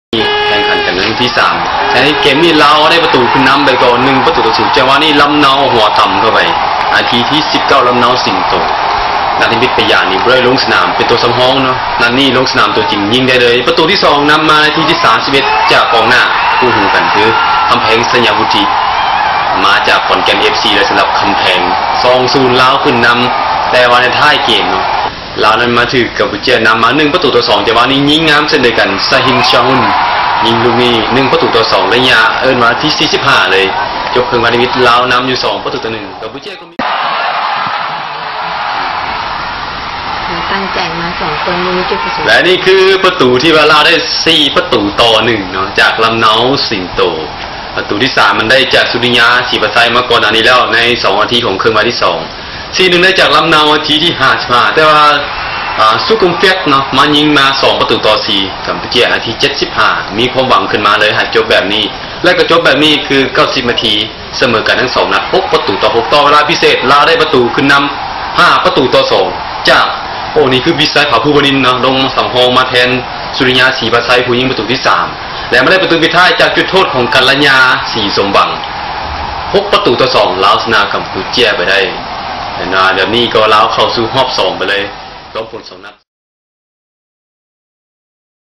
Laos 6 - 2 Cambodia (World Cup 2014 Qualifier Highlights)
Goals scored :
Lamnao SINGTO (LAO) 19', Khampheng SAYAVUTTHI (LAO) 31', Chhin CHHOEUN (CAM) 45', Souliya SYPHASAY (LAO) 47', Lamnao SINGTO (LAO) 55', Kouch SOKUMPHEAK (CAM) 75', Visay PHAPHOUVANIN (LAO) 94', Sysomvang KANLAYA (LAO) 112'